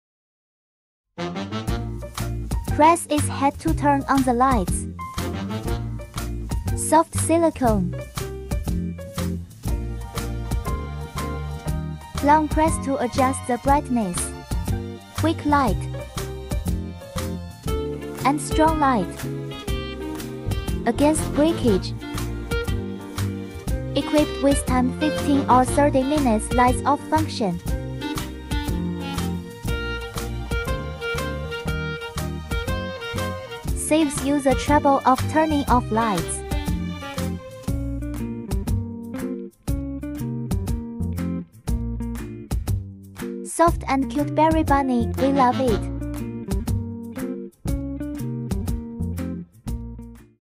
Bunny Night Light | Rabbit Silicone Lamp Soft Gift Bedroom Light
Bunny night light is silicone light with soft warm light and rabbit silicone cover lamp that protect eyes, for children’s bedroom, baby sleeping, relaxing, calming anxiety, home decor, baby nursery.